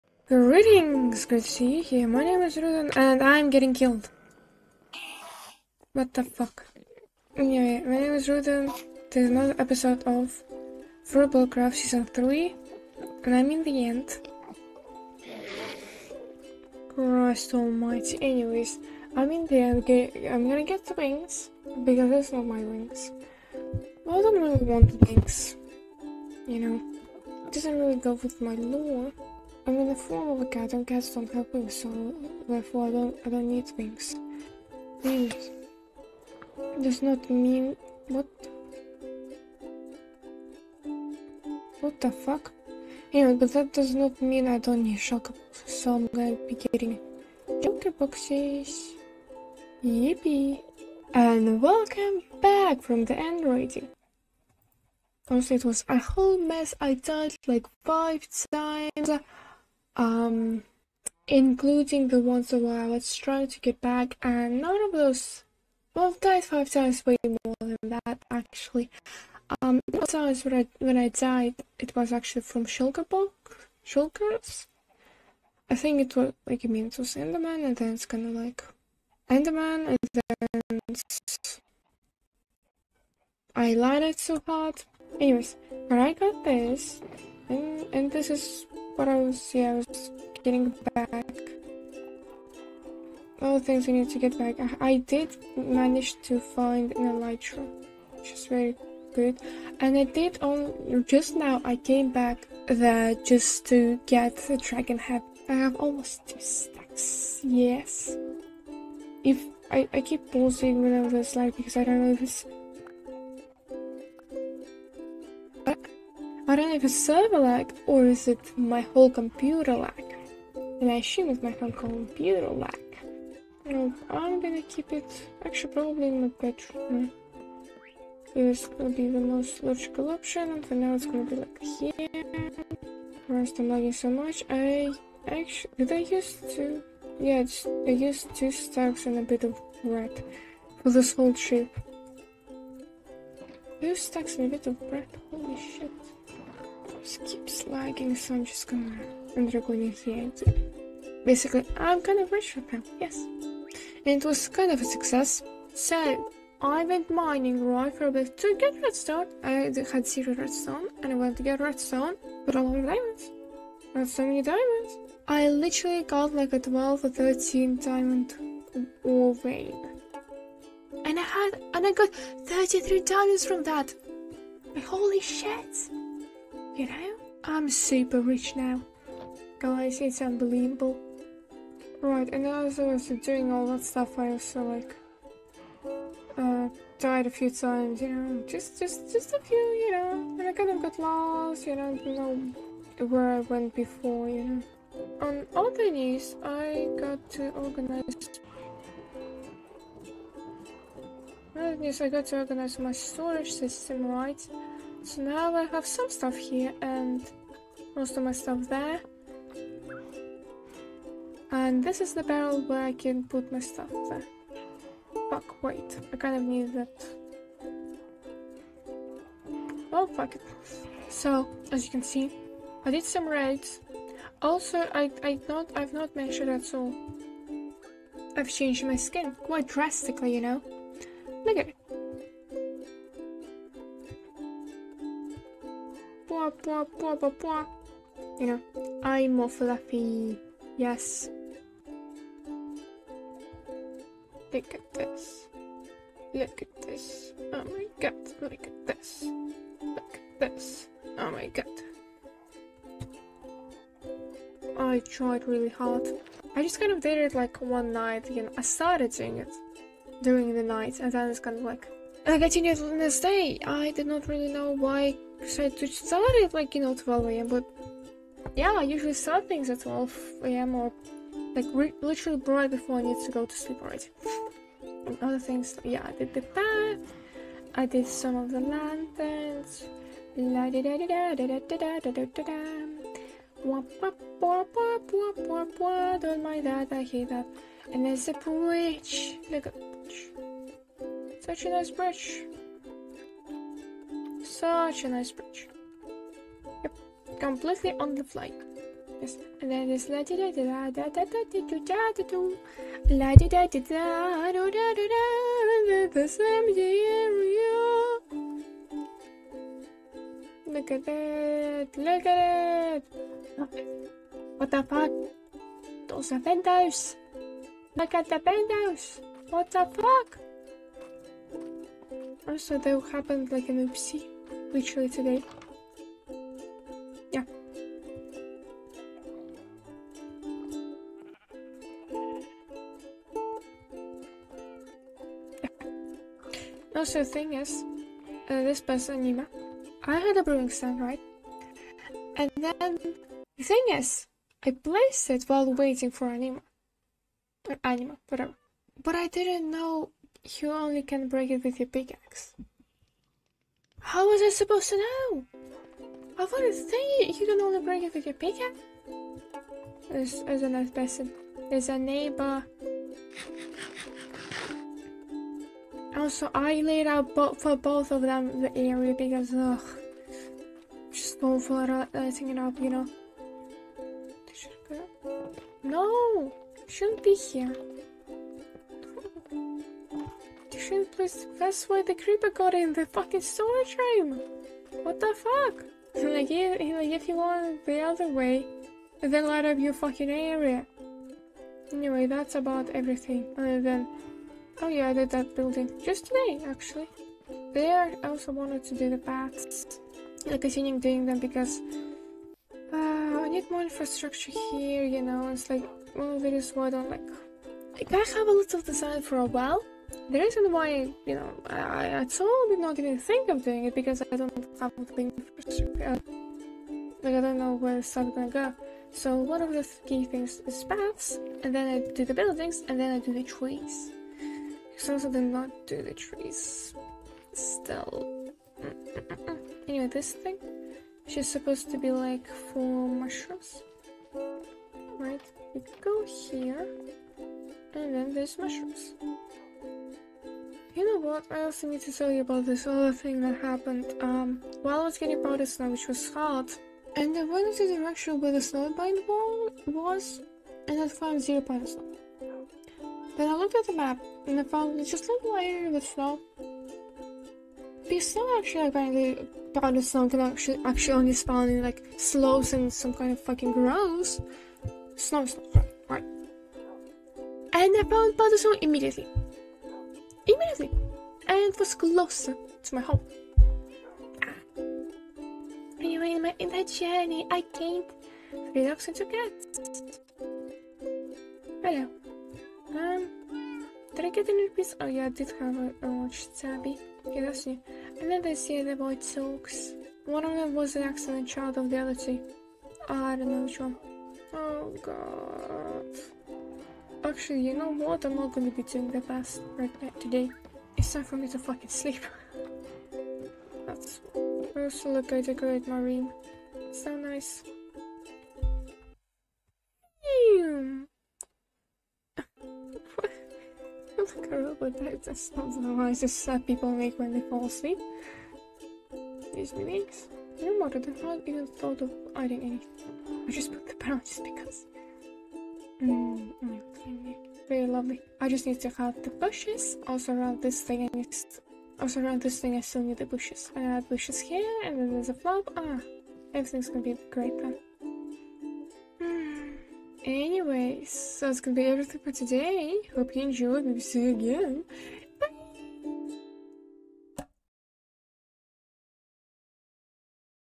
5.Ep of Fruitbowl Craft S3. I'm kinda of rich B)
I went to the end. And then I have wealth, suddenly.

Music by me and my ukulele. Art by me and my digital tablet with stylus.